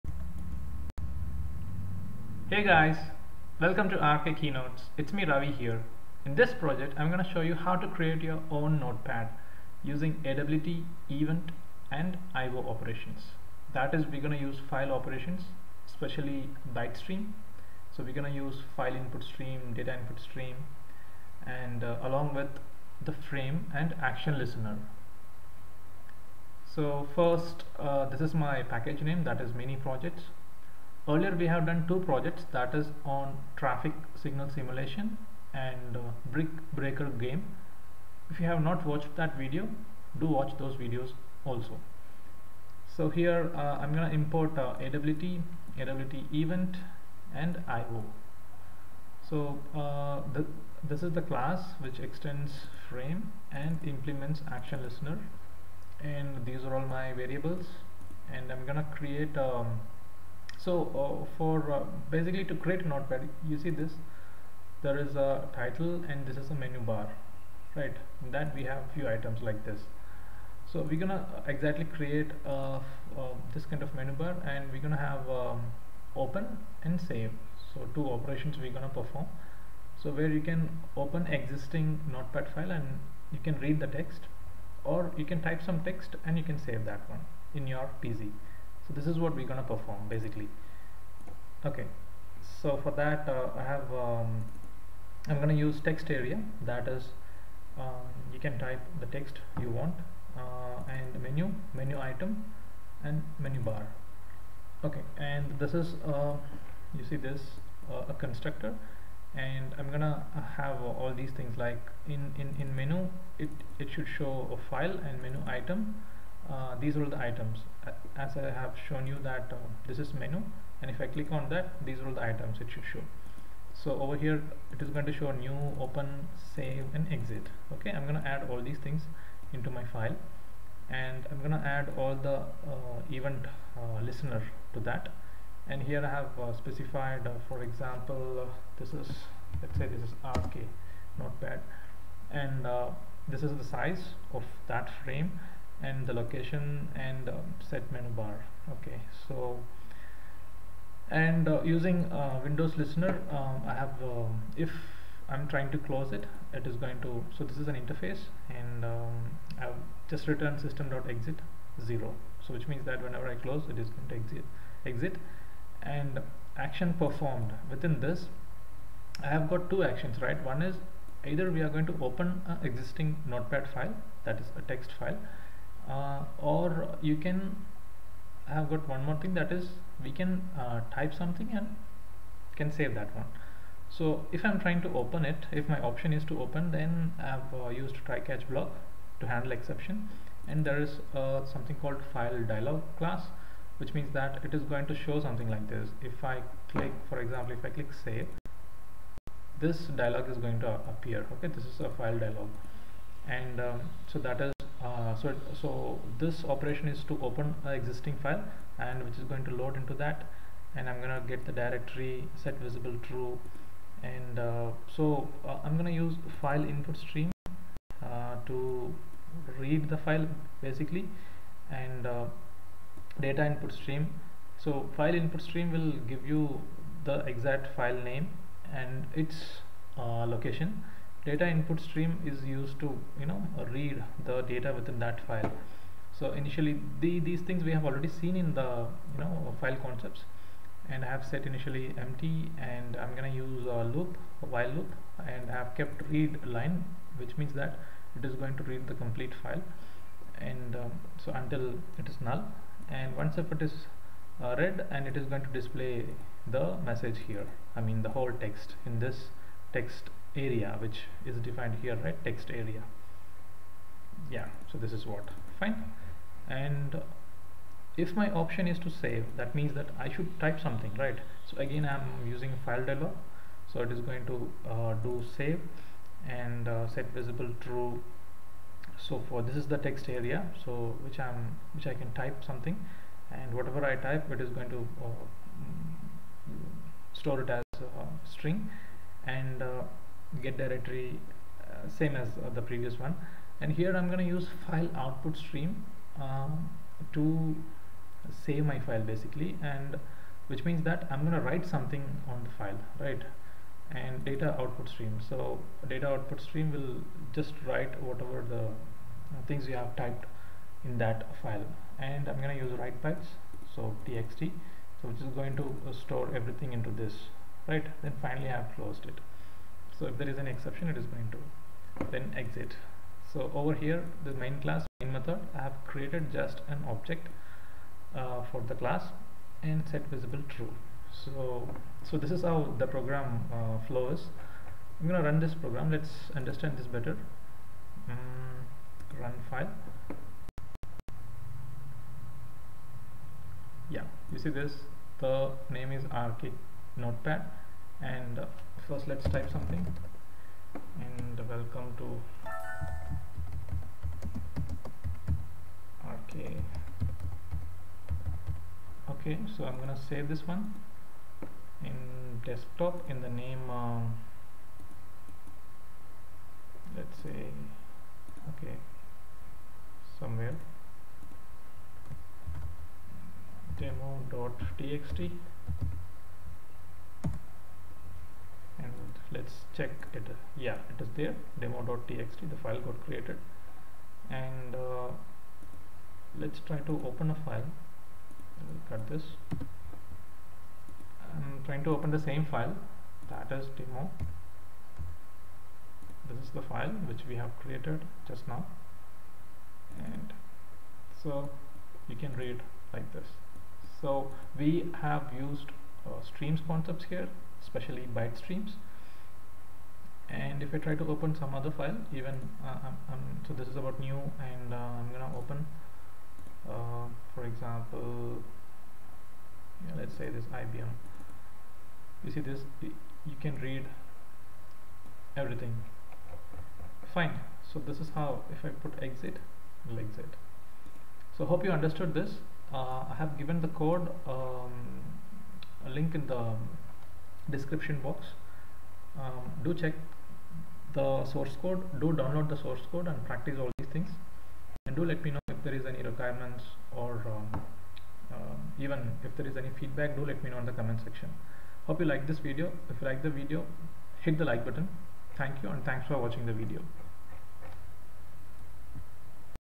#16 (Java Mini Project 03) How to Create Your Own NotePad Using AWT, Event & ActionListener | RK
In this video, I have explained how to create own notepad using AWT, Event & ActionListener in NetBeans IDE.

Regards,
Ravikumar R N